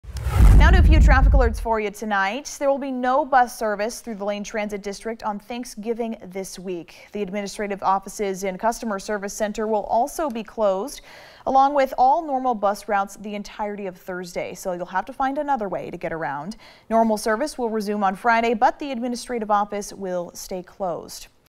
No LTD bus service on Thanksgiving
LTD's administrative offices and customer service office will be closed.